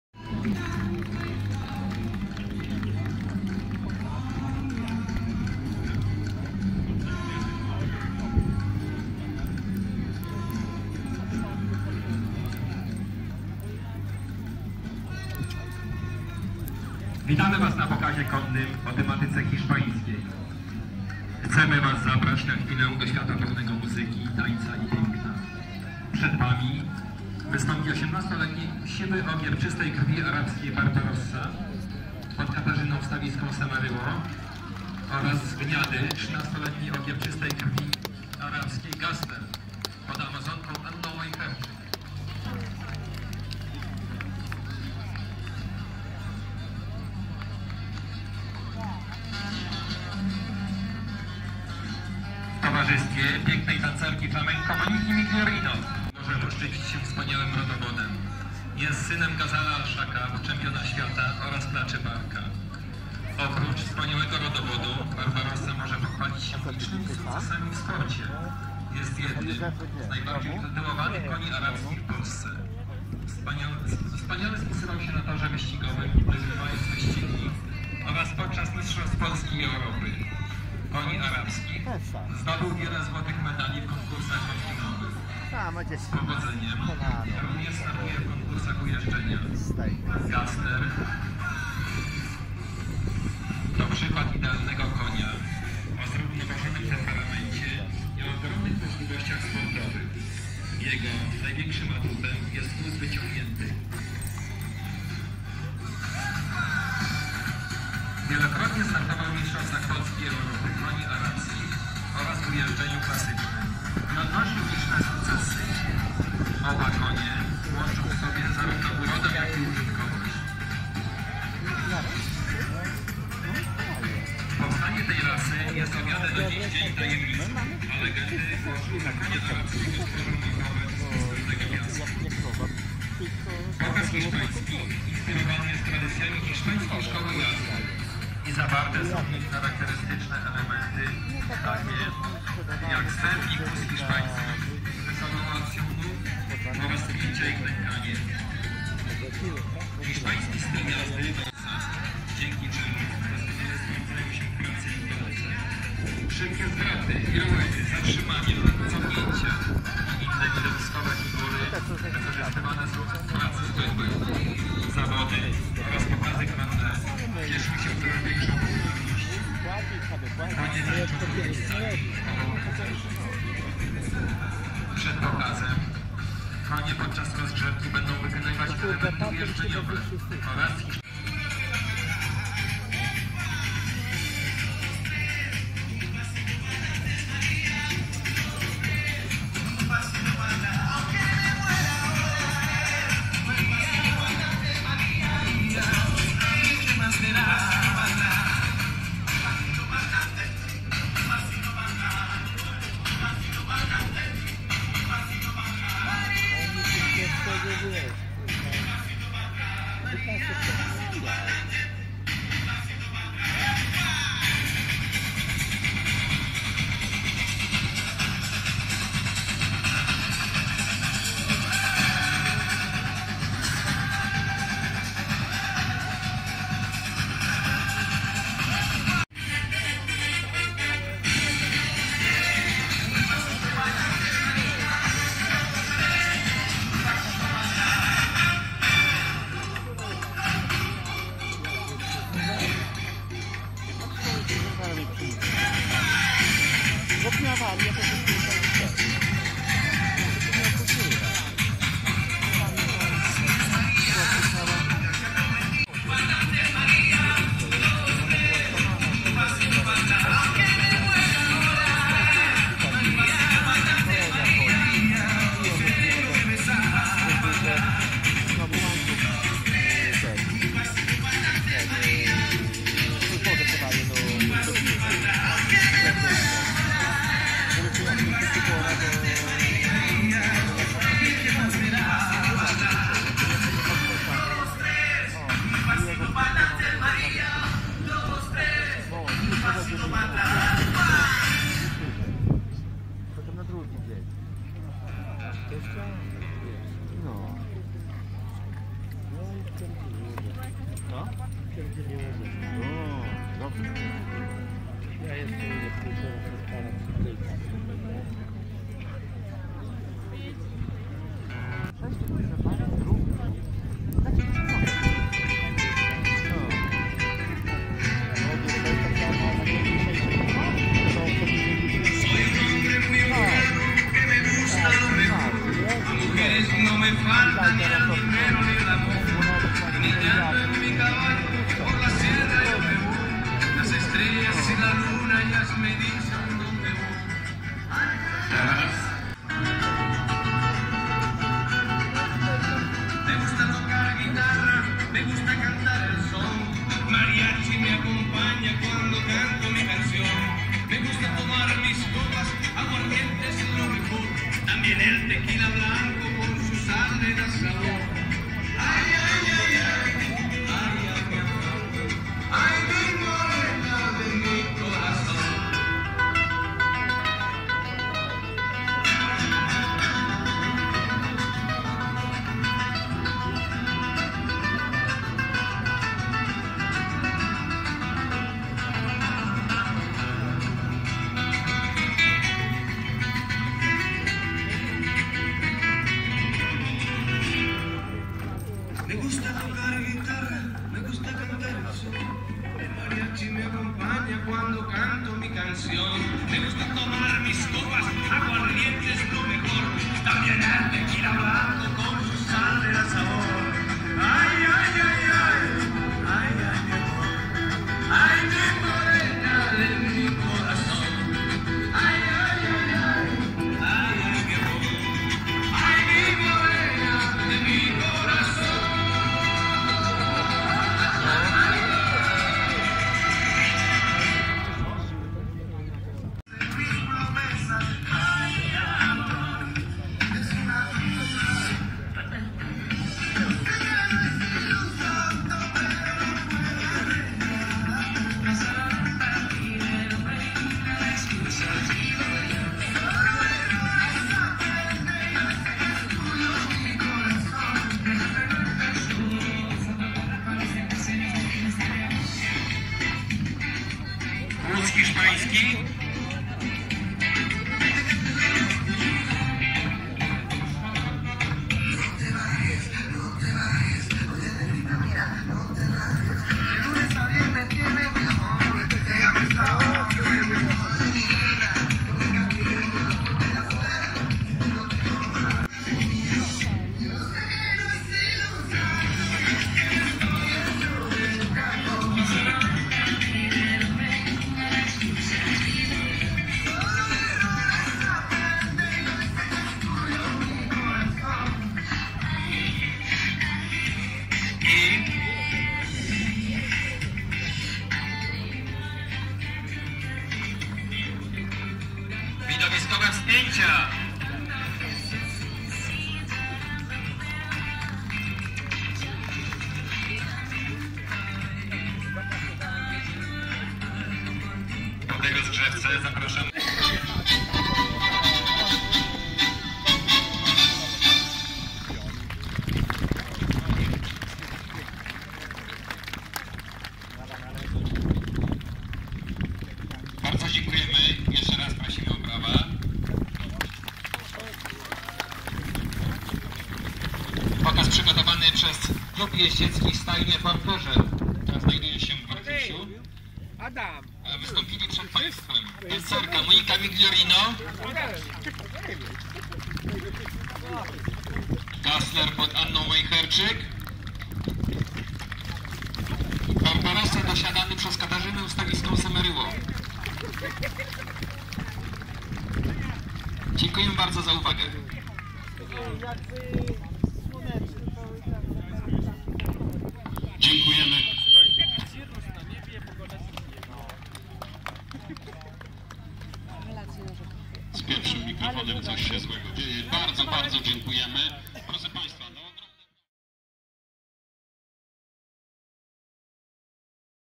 Pokaz jazdy hiszpańskiej - XIII Regionalna Wystawa Koni Zimnokrwistych w Tucznej 2021
Klub jeździecki "For Pleasure" - pokaz jazdy hiszpańskiej

XIII Regionalna Wystawa Koni Zimnokrwistych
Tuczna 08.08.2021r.